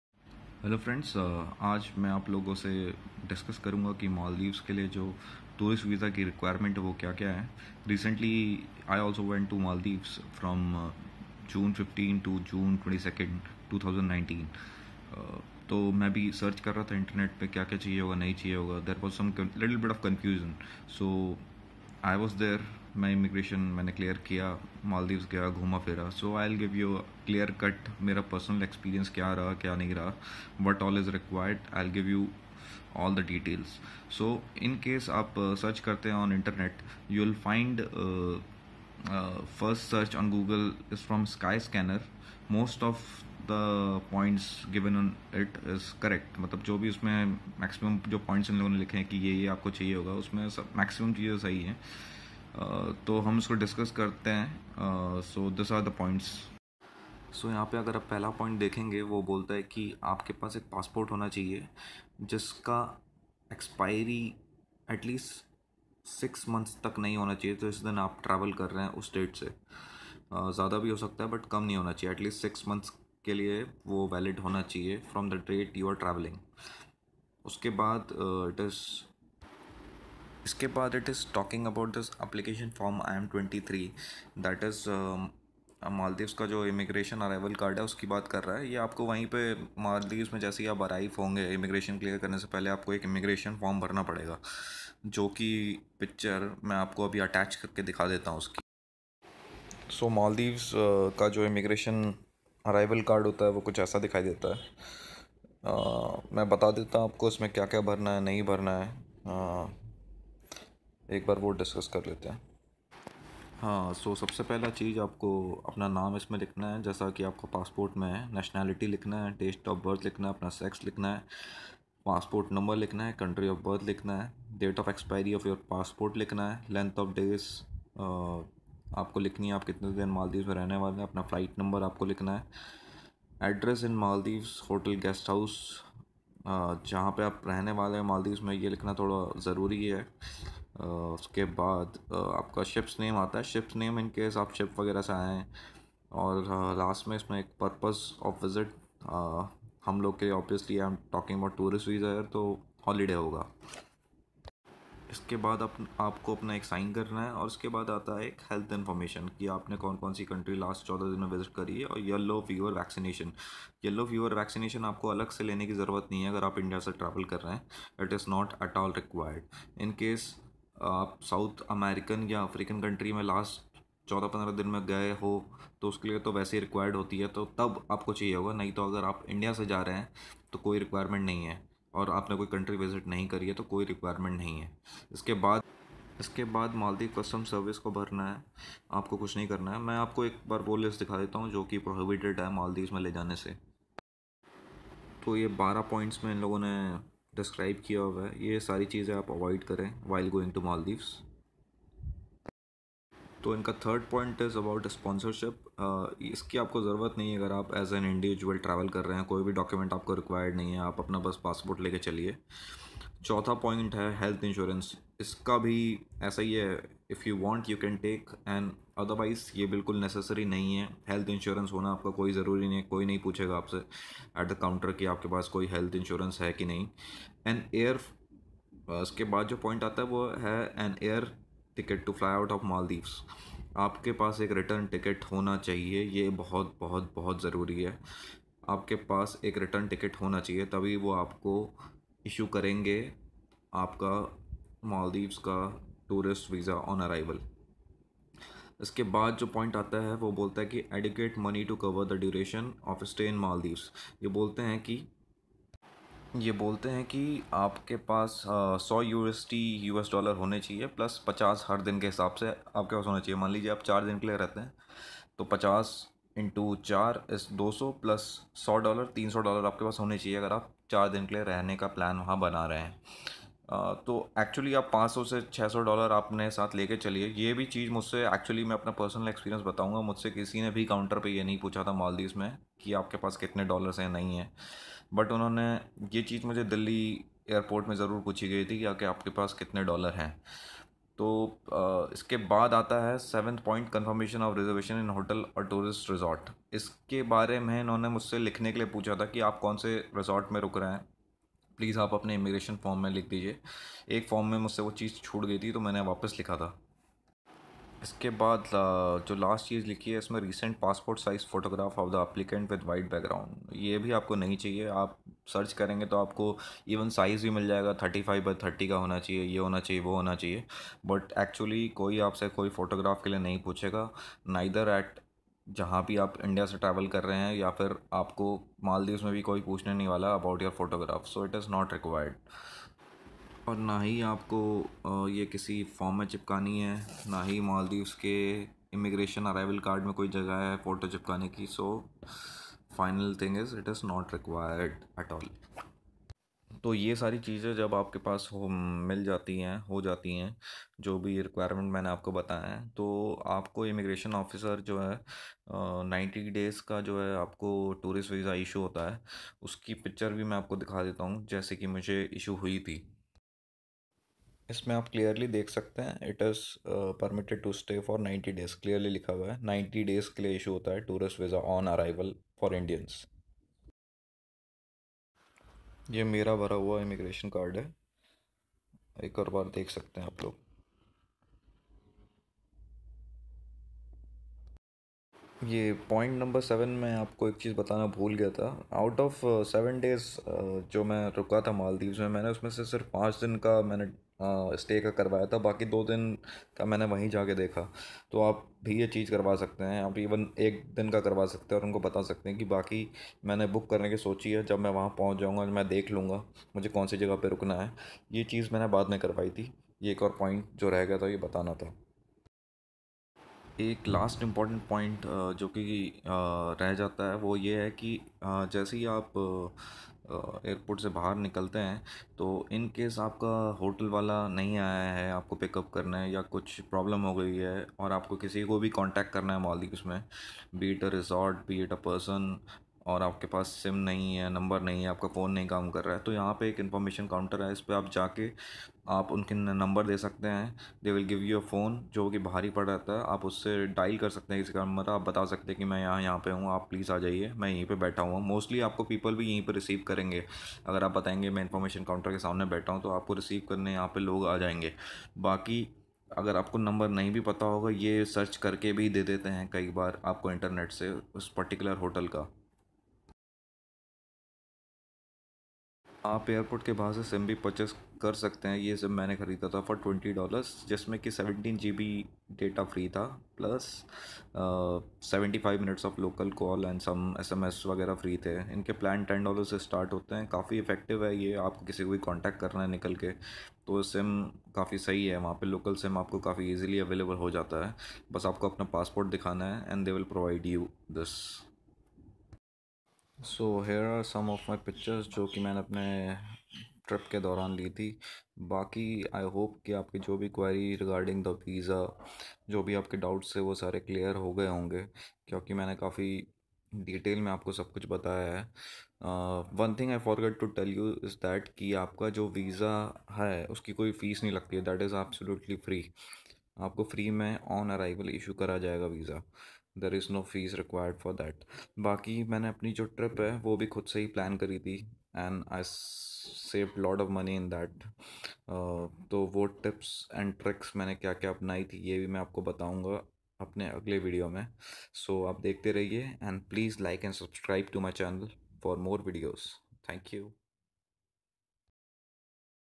Maldives Visa for Indians
In this video , I have discussed in detail tourist visa requirements for maldives.The visa is issued on arrival and is free of charge in case of any other doubt you can leave comment